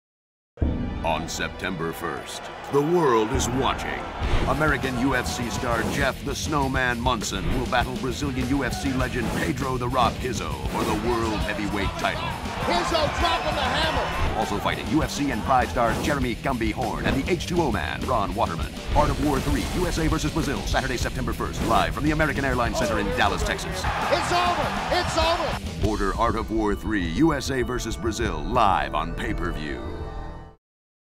Art of War 3 - USA vs. Brazil - 30 Second TV Spot
Television commercial advertising the Art of War 3 - USA vs. Brazil Pay per View MMA promotion. A great show!